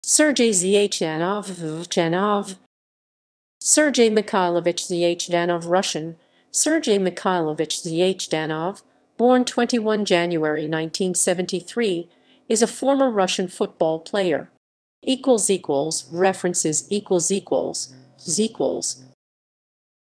Sergei Zhdanov
Sergei Mikhailovich Zhdanov (Russian: Сергей Михайлович Жданов; born 21 January 1973) is a former Russian football player.

== References ==